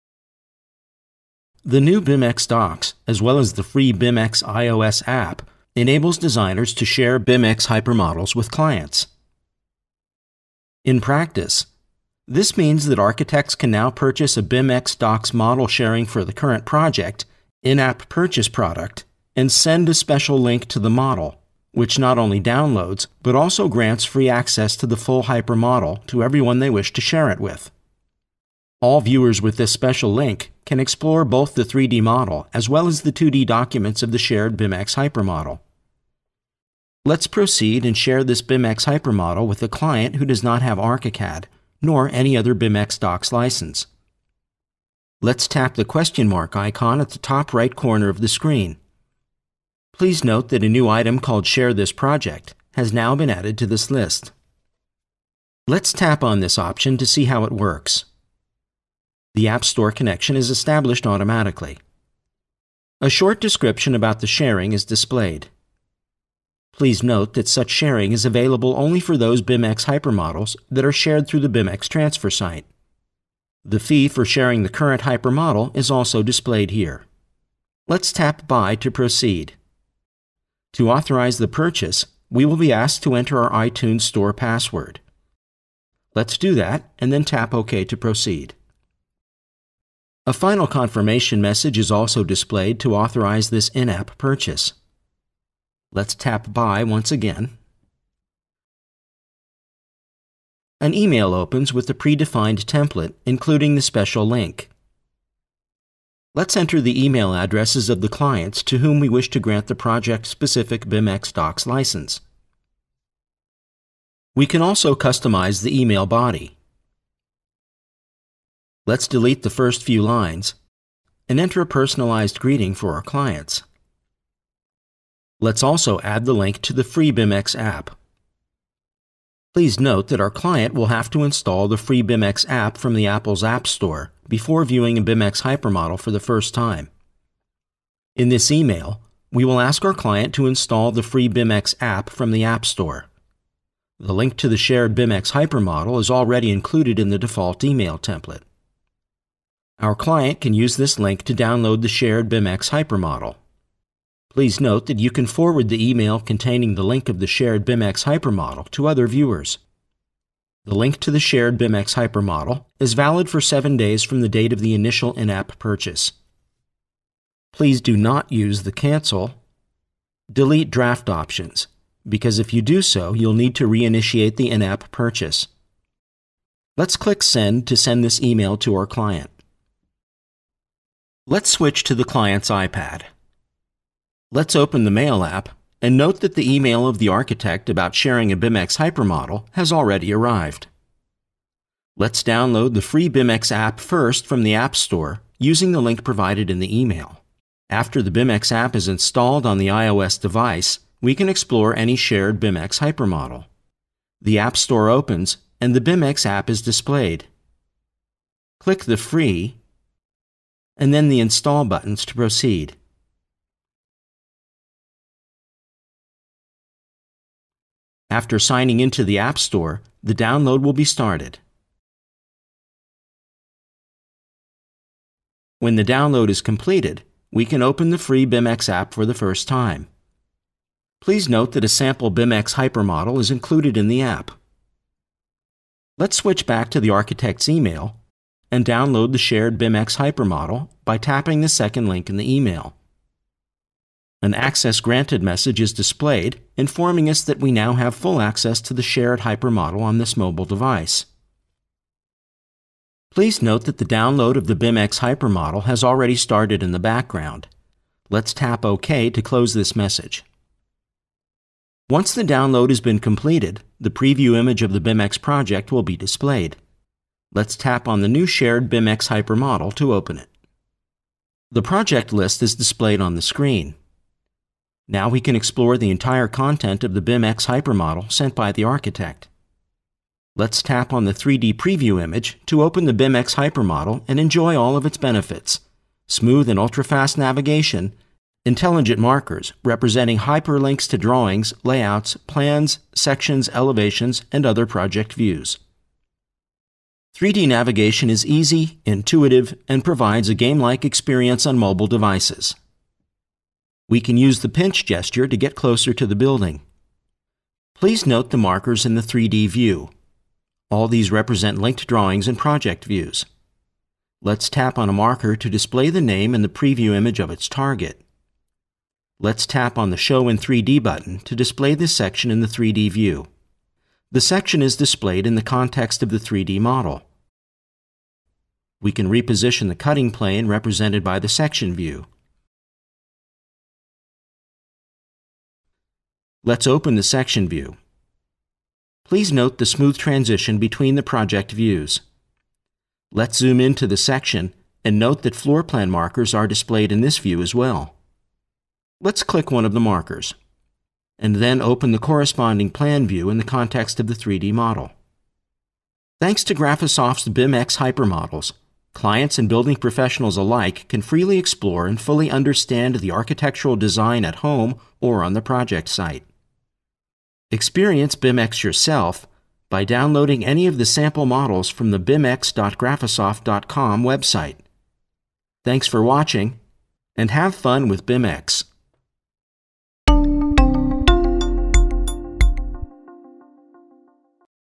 BIMx - Sharing BIMx Hyper Models with Clients
The new BIMx Docs - as well as the free BIMx - iOS App enables designers to "share BIMx Hyper-Models with clients.

All viewers with this link can explore both the 3D model as well as the 2D documents of the shared BIMx hyper model.